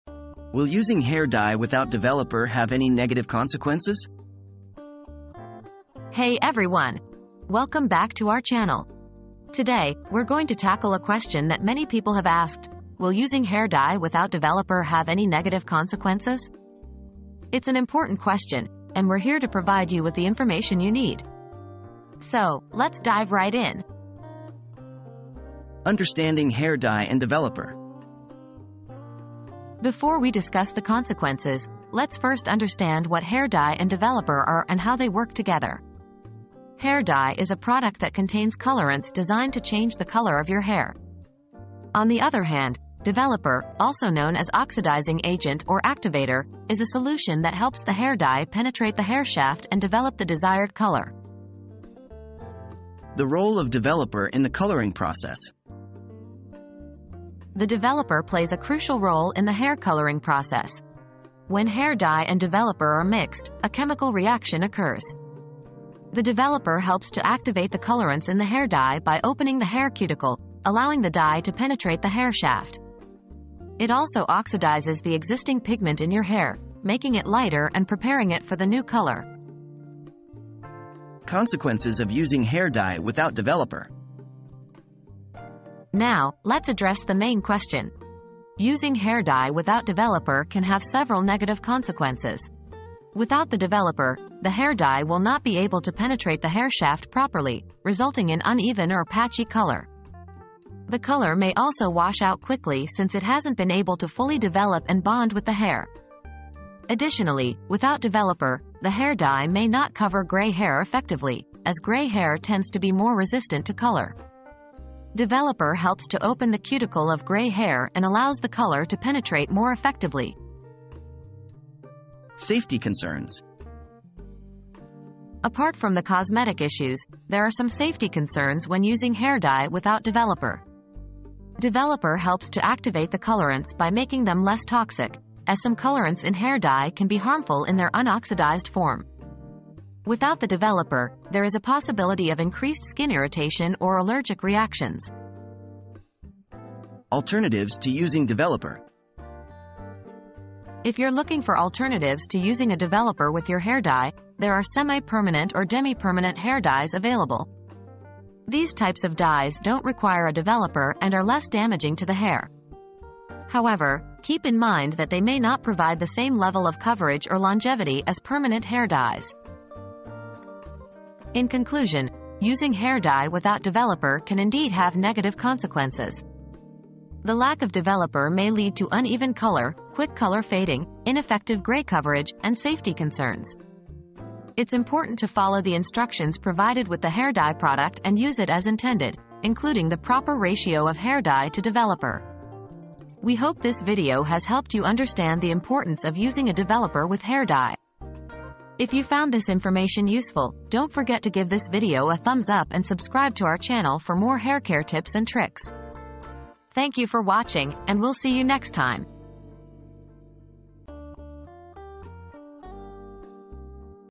Will using hair dye without developer have any negative consequences?
Will Using Hair Dye Without Developer Have Any Negative Consequences? • Will Using Hair Dye Without Developer Have Any Negative Consequences? Find out now! Learn how hair dye and developer work together and the consequences of skipping the developer in this informative video. Don't risk uneven color or quick fading – watch now and get all the information you need to dye your hair safely and effectively!

00:00 • Will using hair dye without developer have any negative consequences?
00:25 • Understanding Hair Dye and Developer
00:53 • The Role of Developer in the Coloring Process
01:21 • Consequences of Using Hair Dye Without Developer
02:02 • Safety Concerns
02:27 • Alternatives to Using Developer